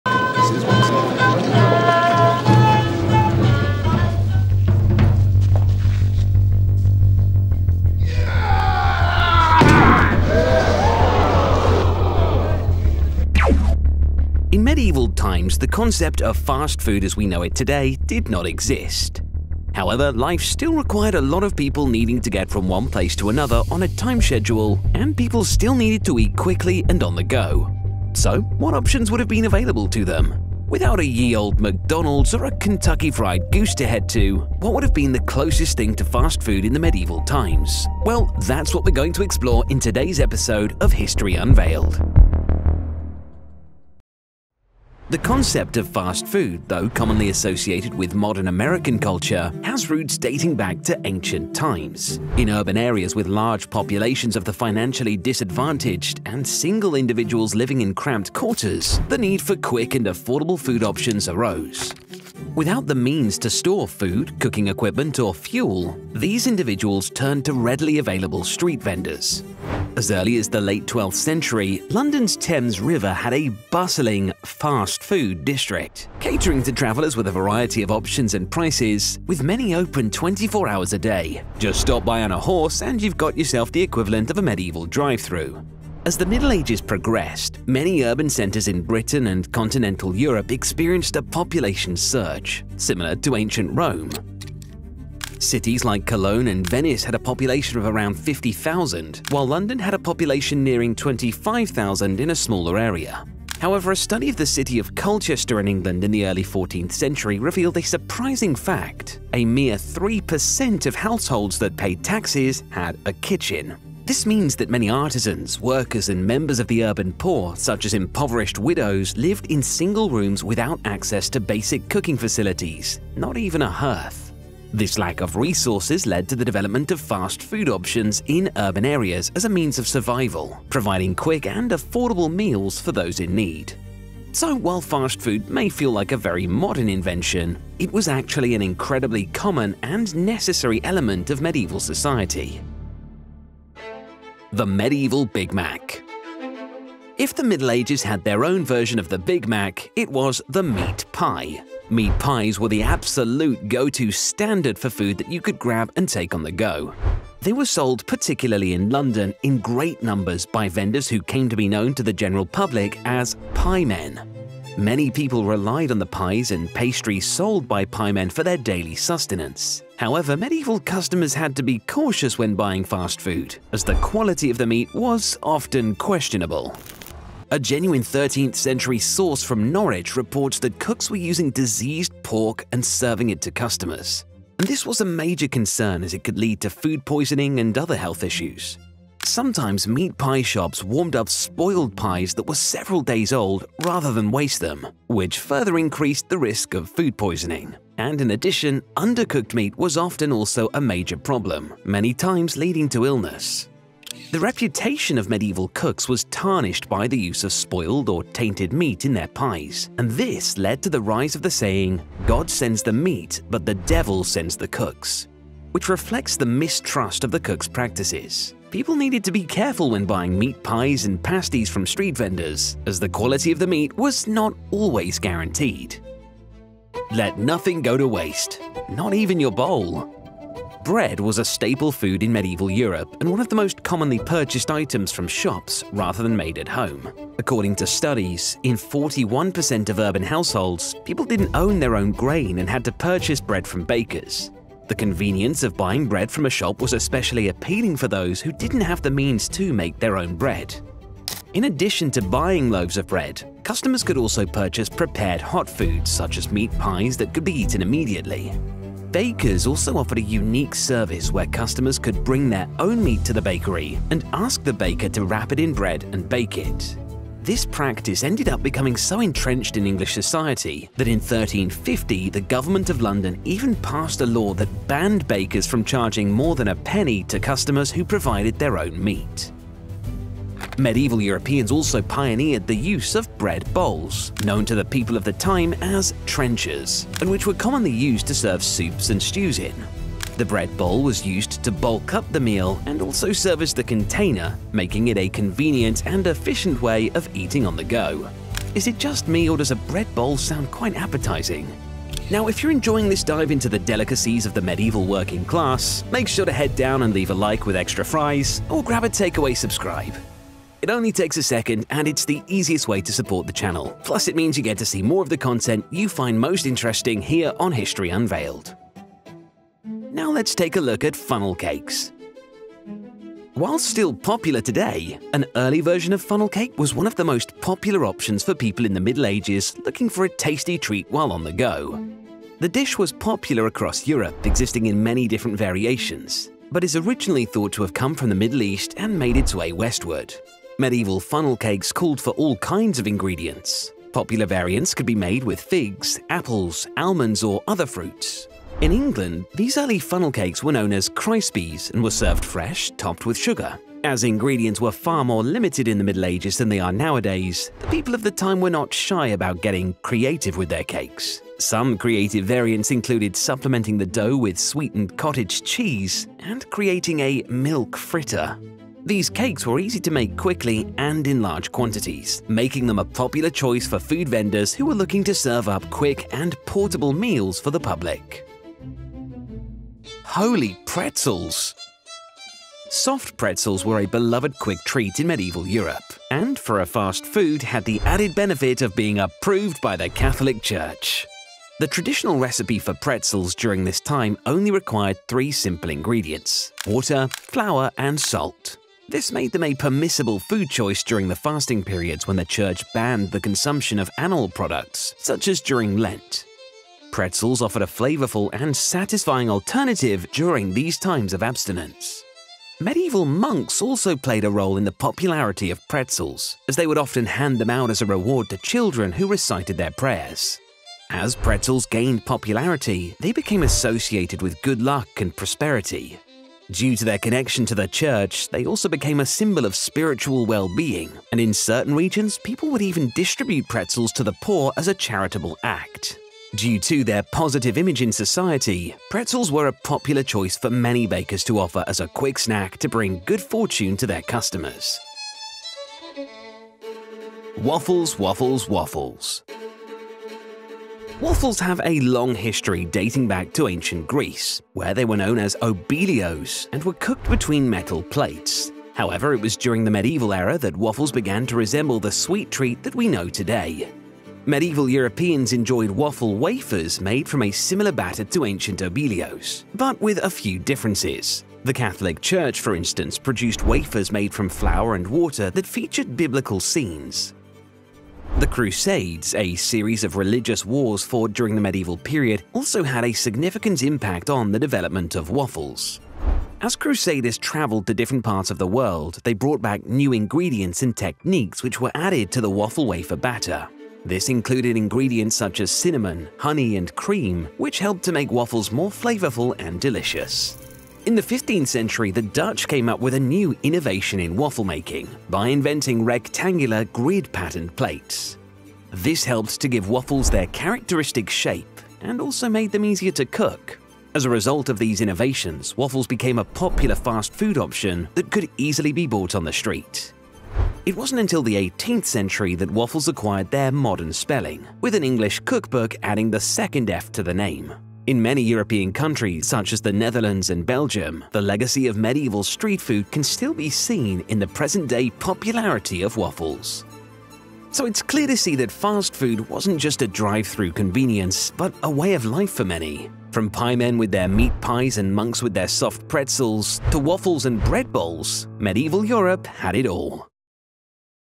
Medieval Junk Food: Delicious or Disgusting?
Without modern-day fast-food chains, people still needed to eat quickly and on the go. From meat pies to bread bowls, vendors provided a variety of options to suit the needs of customers. However, caution was advised when buying fast food as the quality of meat used was often questionable. The episode also explores the use of bread bowls and the early version of funnel cakes. Join us on this fascinating journey through the delicacies of the medieval working class.